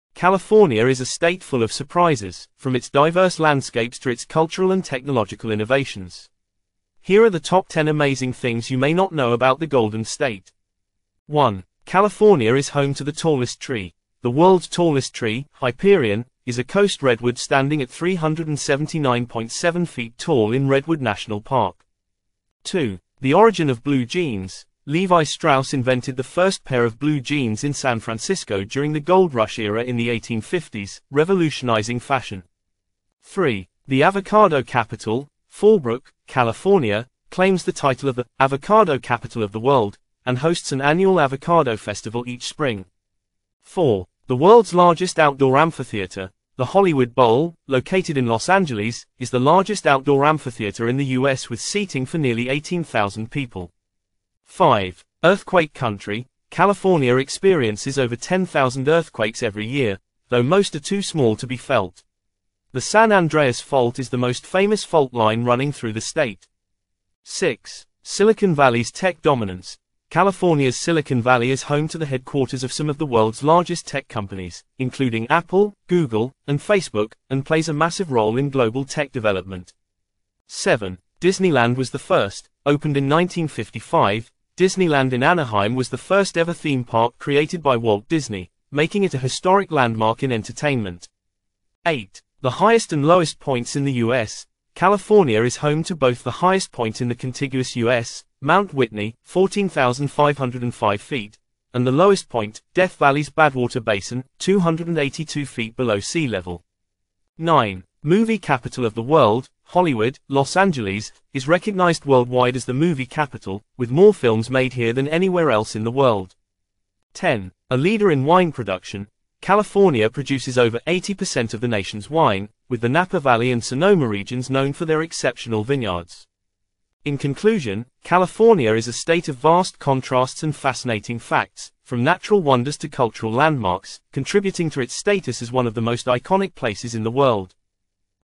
Top 10 Amazing Things You Didn’t Know About California
California is full of surprises! Check out our Top 10 Amazing Things You Didn’t Know About California and discover incredible facts about the Golden State that will blow your mind. From hidden gems to record-breaking landmarks!